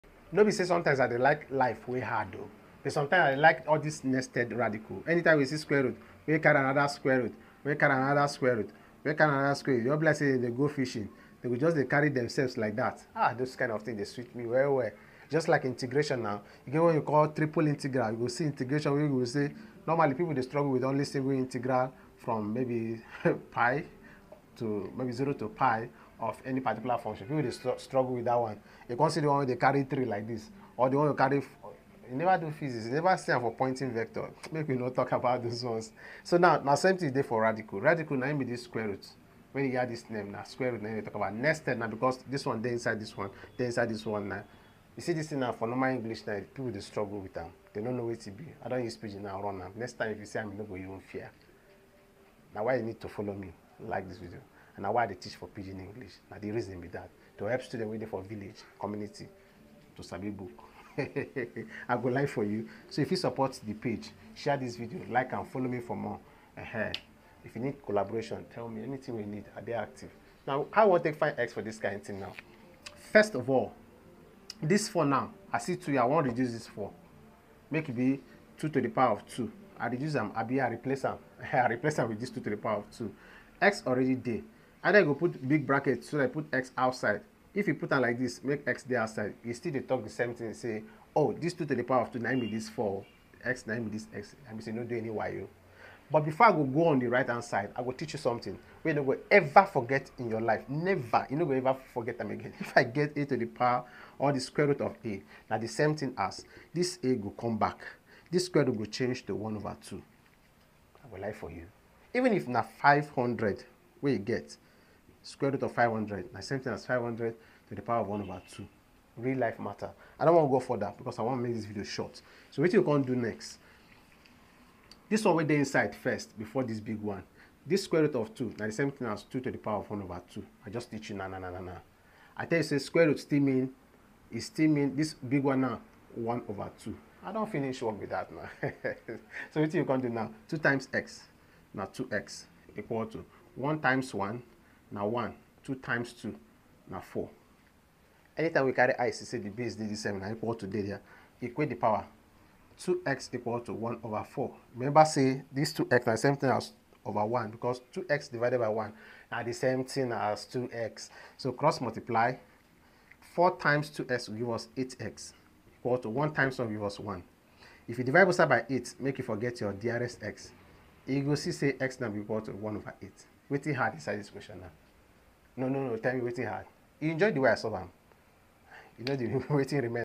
Nested Radical Made Easy
wetin be x? 😅

This one dey sweet because e go test whether you sabi indices and root matter!

Make we break am down 👇
We know say √√2 = 2^(1/4)
And 4 = 2²
So e go be (2²)^x = 2^(1/4)
Join the powers → 2^(2x) = 2^(1/4)

Meaning say 2x = 1/4
So x = 1/8 ✅

Math don soft be that 😎
If you understand this one, drop Nested Radical don clear!

And if you still dey confuse, drop your question for comment make we solve am together.

This same type of question fit appear for IGCSE, GCSE, ASVAB, and even WAEC!

Share am make another person learn too 💪